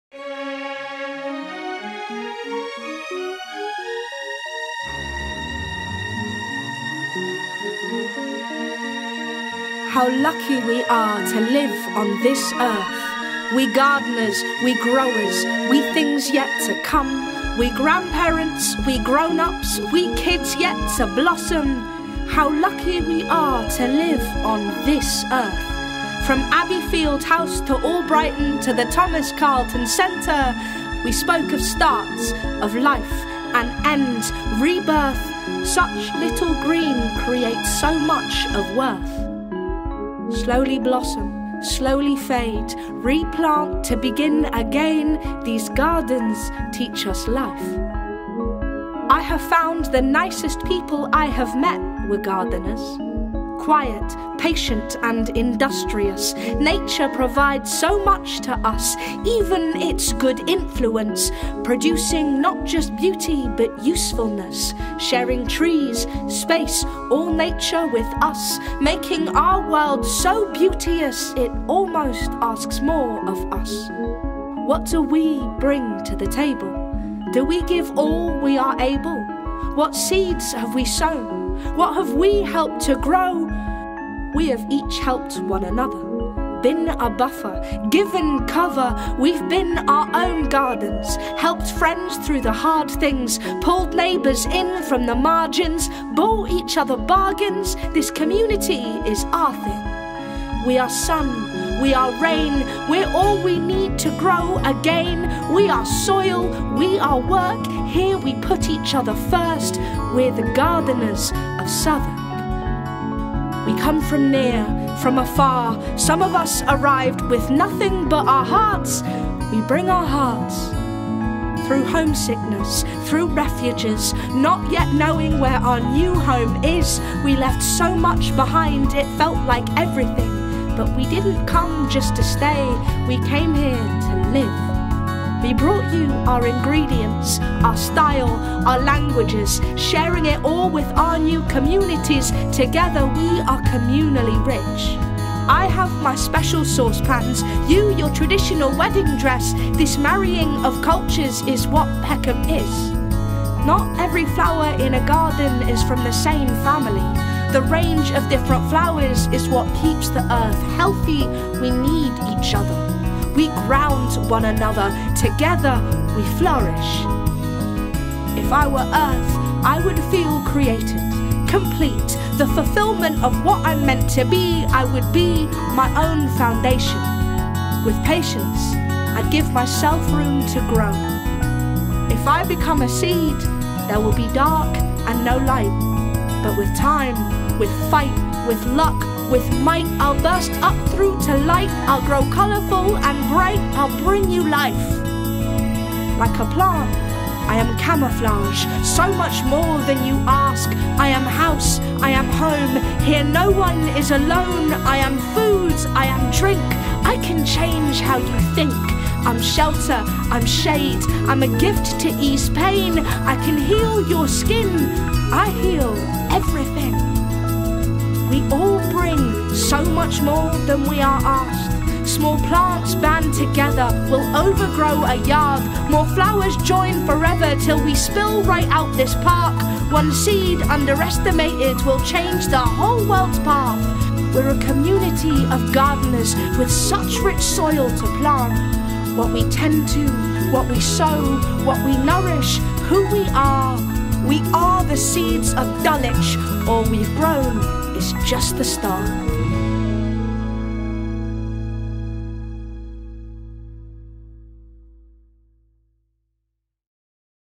Spring Rising Poem
A video made by Output Arts to accompany the poem written by Sophia Walker for the Spring Rising event, co-produced by Emergency Exit Arts and Output Arts, commissioned by Southwark Presents with funding from Southwark Council and Arts Council England.

Thanks to St John's and St Clement's Primary School year 5, Link Age Southwark, Albrighton Community Centre's youth club and Southwark Adult Learning's ESOL class for hosting us and contributing their ideas to the poem and their faces to the video.

Voice: Sophia Walker
Additional footage: Emily Taylor of brothersister